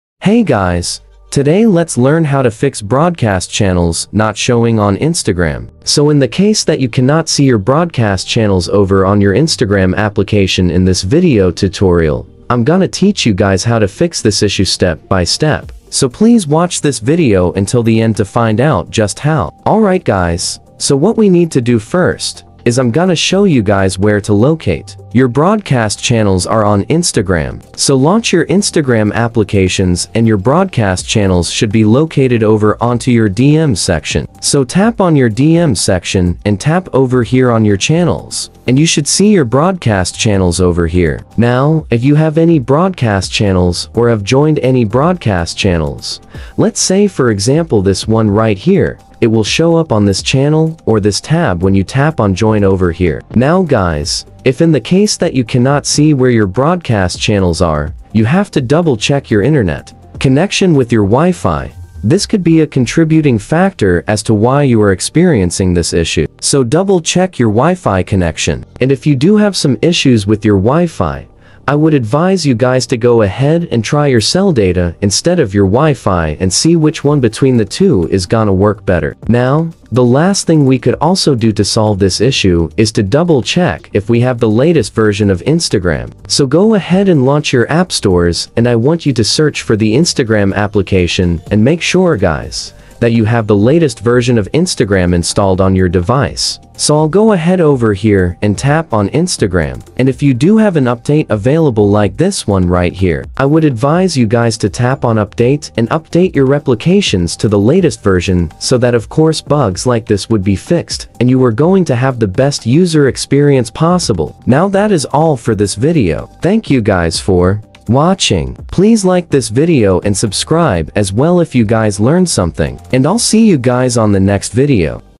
How To Fix Broadcast Channel Not Showing On Instagram [easy]
"📢 Having trouble with your Instagram broadcast channel not showing up? Fret not! In this easy-to-follow tutorial, we'll walk you through the steps to troubleshoot and fix the issue, so you can get back to broadcasting on Instagram seamlessly.

From checking settings to potential app updates, we'll cover everything you need to know to make sure your broadcast channel is visible and accessible to your followers.

Let's get your broadcast channel back in the spotlight! 🌟🔧 Instagram BroadcastChannel TechFix"